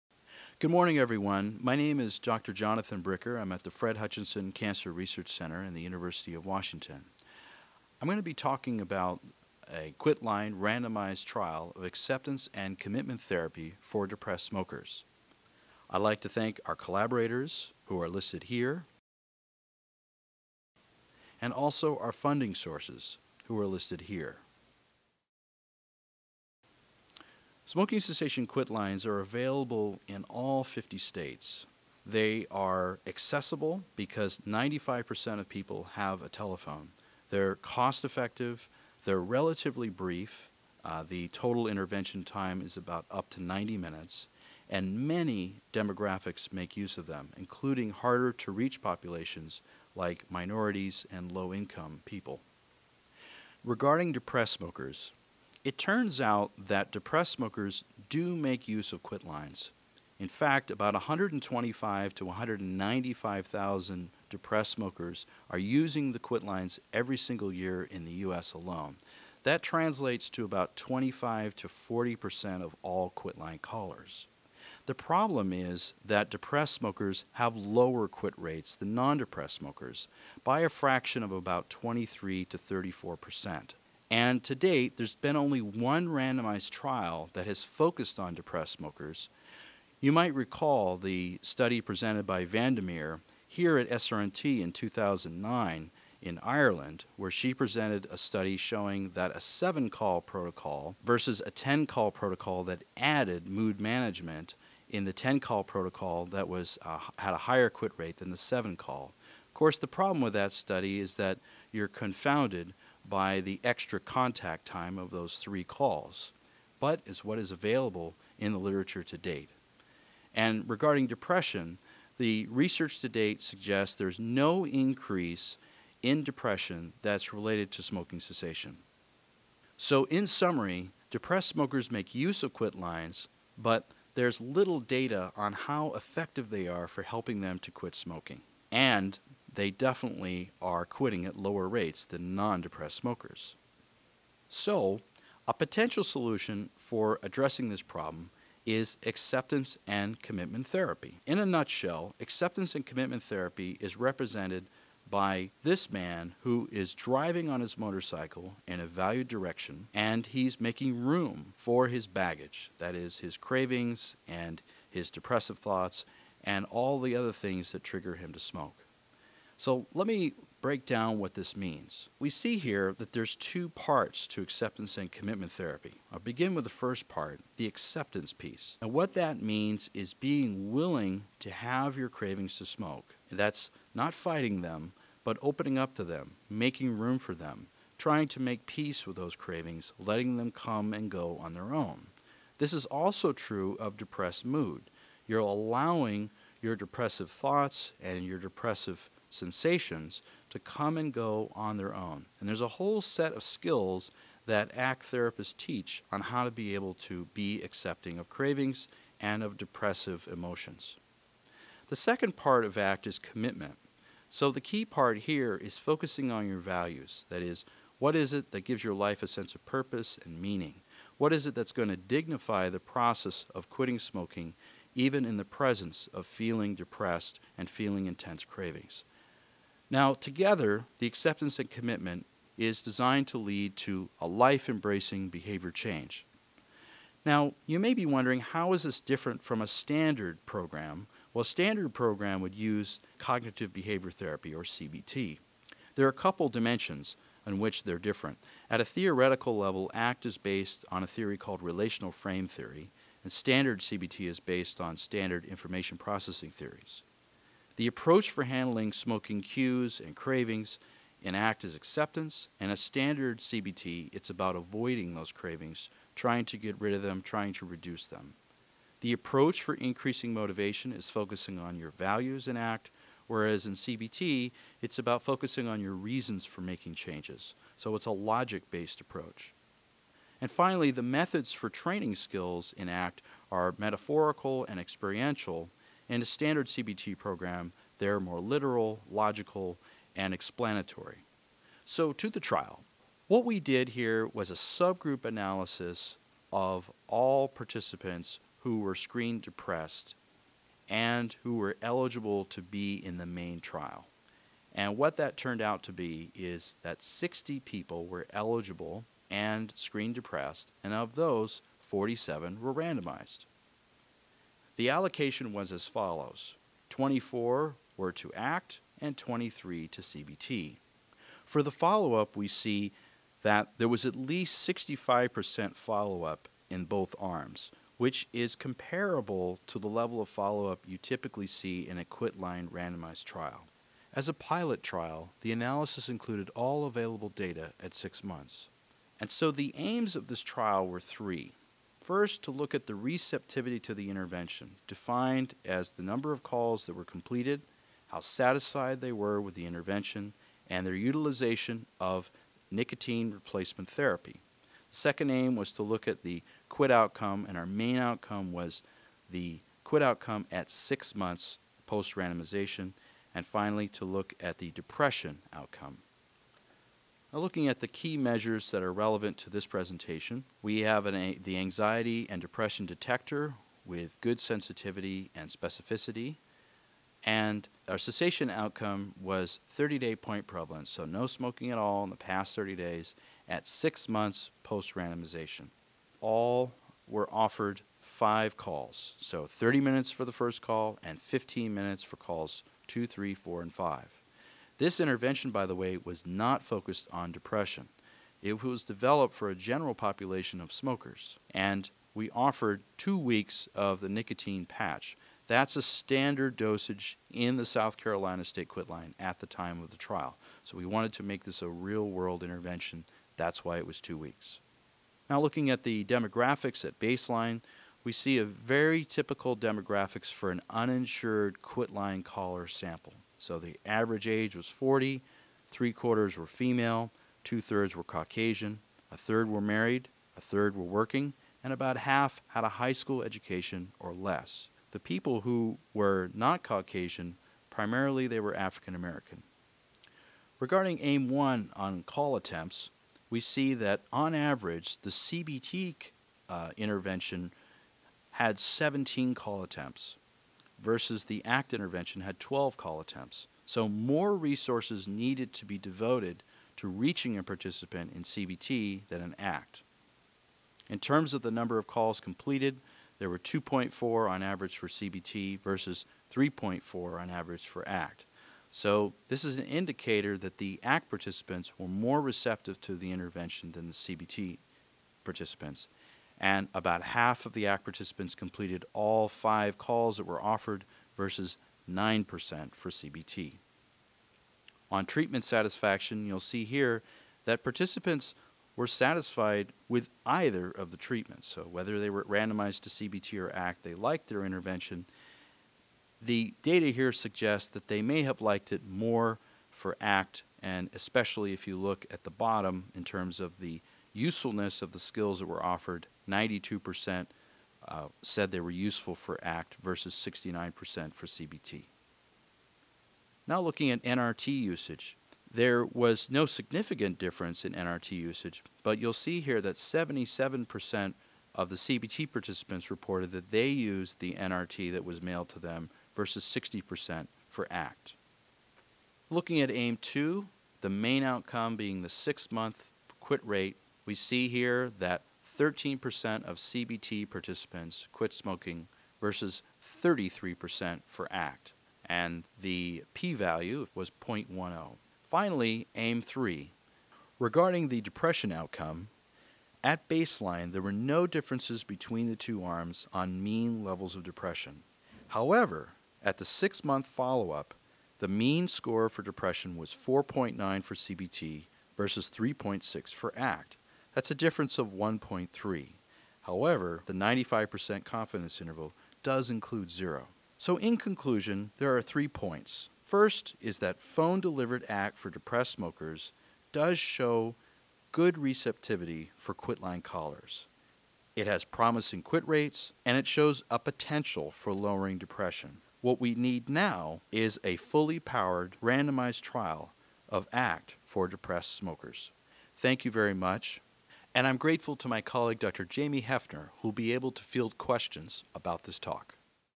Acceptance and Commitment Therapy for Depressed Smokers
Presented at the Society for Research on Nicotine and Tobacco (SRNT.org) 21st annual conference, February 28, 2015, Philadelphia, PA.